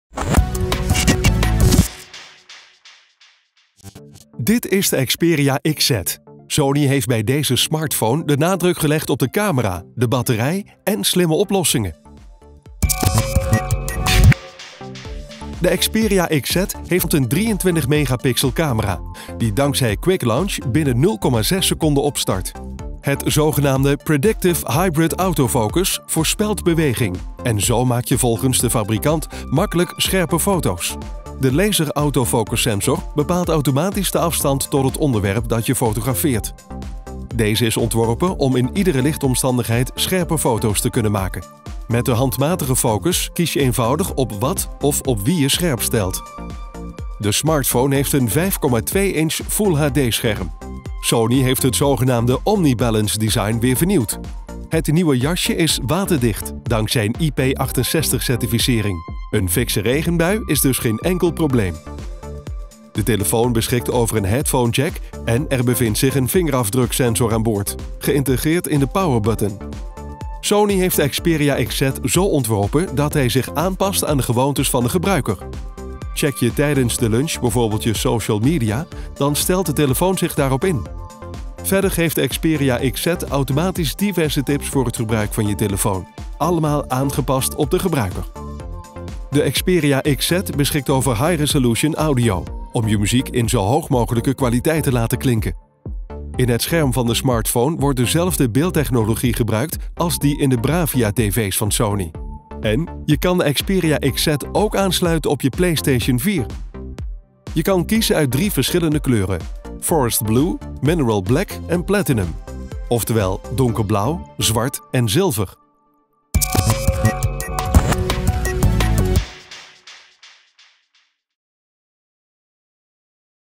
Sony Xperia XZ - EverythingVideo
Dit is de Xperia XZ. Sony heeft bij deze smartphone de nadruk gelegd op de camera, de batterij en slimme oplossingen.

De Xperia XZ heeft aan de voorkant een 23 megapixel camera, die dankzij Quick Launch binnen 0,6 seconden opstart.

Het zogenaamde ‘Predictive Hybrid Autofocus’ voorspelt beweging, en zo maak je volgens de fabrikant makkelijk scherpe foto’s. De ‘Laser Autofocus Sensor’ bepaalt automatisch de afstand tot het onderwerp dat je fotografeert. Deze is ontworpen om in iedere lichtomstandigheid scherpe foto’s te kunnen maken.

De sluitertijd kan worden aangepast, van 1/8e van een seconde tot 1/4000e. Met de handmatige focus kies je eenvoudig op wat, of op wie, je scherpstelt.

Je kan kiezen uit drie verschillende kleuren: Forest Blue, Mineral Black en Platinum, oftwel: Donkerblauw, zwart en zilver. De smartphone heeft een 5’2” inch Full HD scherm.

Sony heeft het zogenaamde ‘omnibalance design’ weer vernieuwd. Het nieuwe jasje is waterdicht dankzij een IP68 certificering, een fikse regenbui is dus geen enkel probleem! De telefoon beschikt over een headphone jack en er bevindt zich een vingerafdruksensor aan boord,.. geïntegreerd in de power-button.

De Xperia XZ beschikt over High Res Audio om je muziek in zo’n hoog mogelijke kwaliteit te laten klinken. In het scherm van de smartphone wordt dezelfde beeldtechnologie gebruikt als die in de Bravia TV’s van Sony. En gamers opgelet, je kan de Xperia XZ ook aansluiten op je PlayStation 4. Zo kun je, ook als de TV bezet is, gewoon door-gamen.